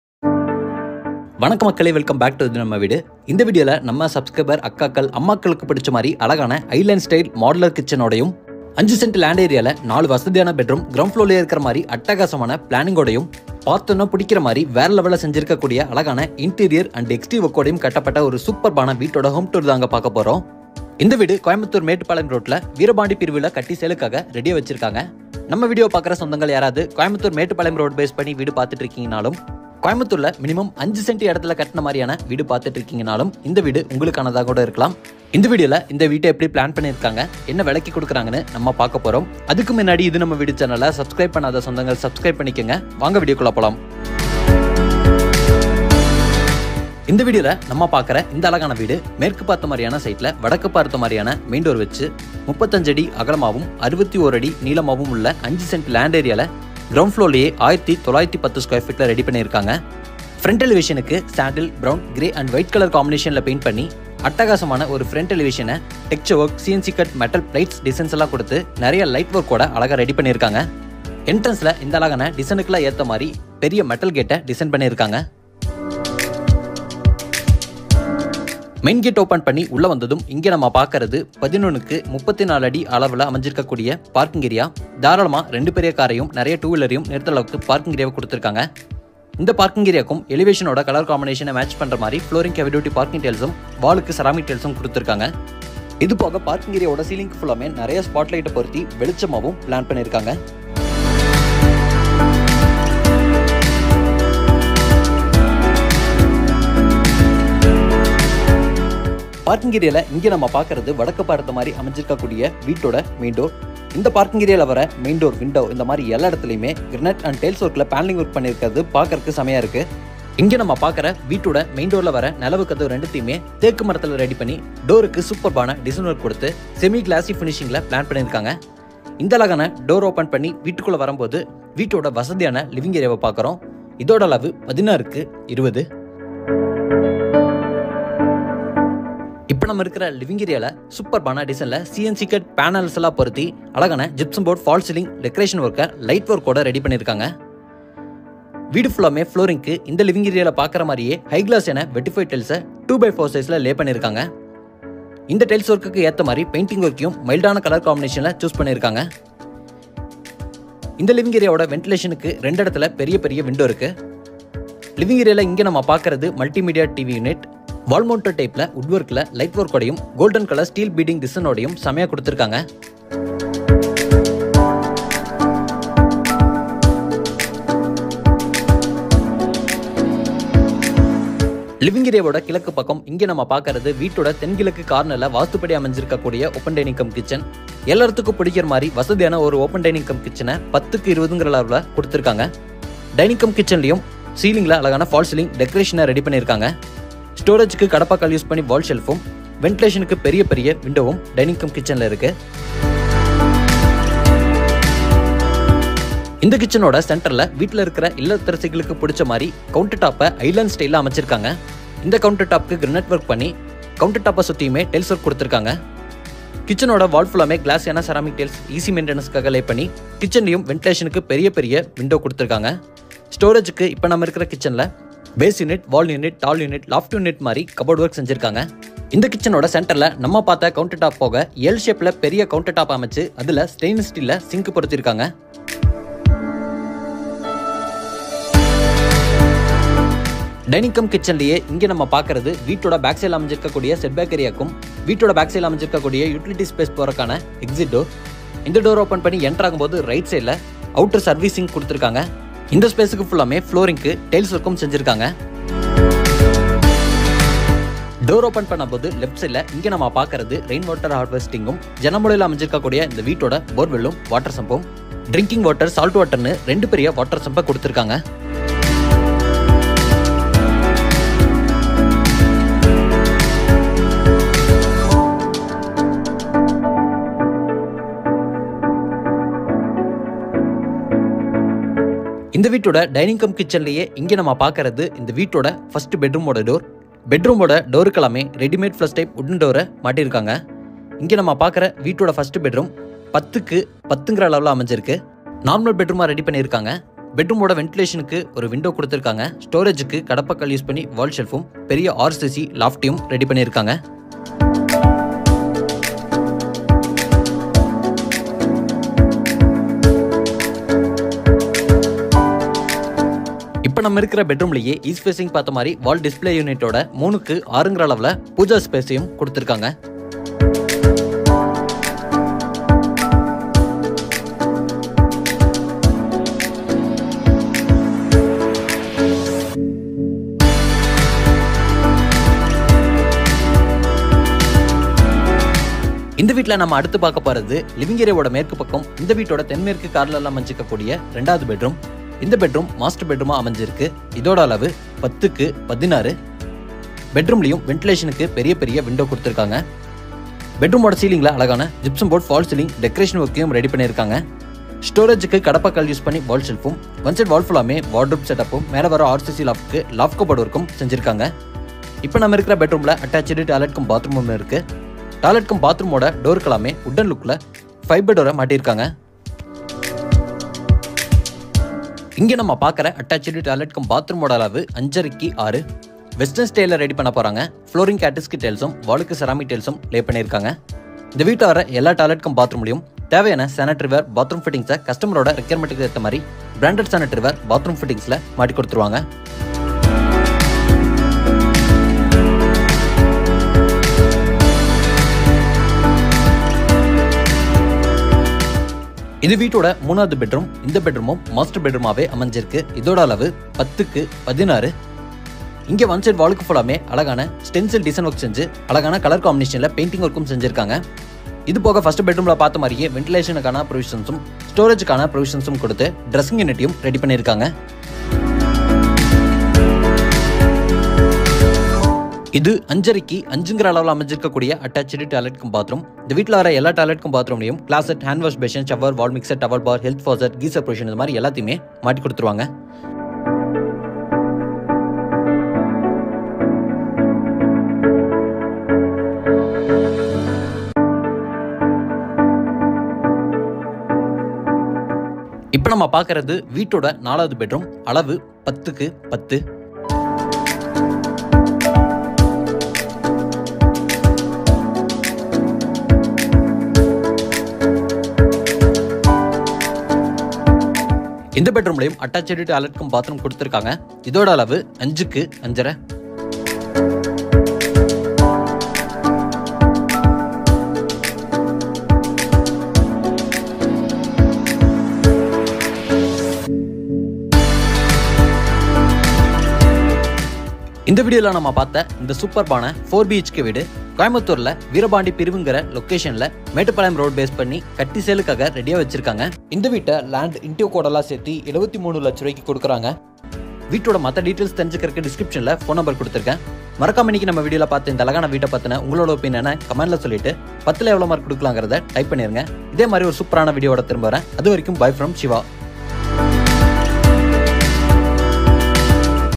5 சென்டில் 1910 sqft ல் கட்டப்பட்ட அழகிய 4BHK வீடு விற்பனைக்கு | House for sale in Veerapandi pirivu
5 சென்டில் 1910 sqft ல் கட்டப்பட்ட அழகிய 4BHK வீடு விற்பனைக்கு | House for sale in Veerapandi pirivu

PROPERTY DETAILS

75% LOAN AVAILABLE

GROUND FLOOR

▪️Car parking : 11' × 33'
▪️Living hall : 16' × 20'
▪️Kitchen & Dining : 10' × 20'
▪️Pooja room : 3' × 6'
▪️Attached bathroom : 5'6" × 6'
▪️3rd bedroom : 10' × 16'
▪️Attached bathroom : 5'5" × 5'
▪️4th bedroom : 11' × 10'
▪️Utility

Location -

Veerapandi pirivu, Coimbatore.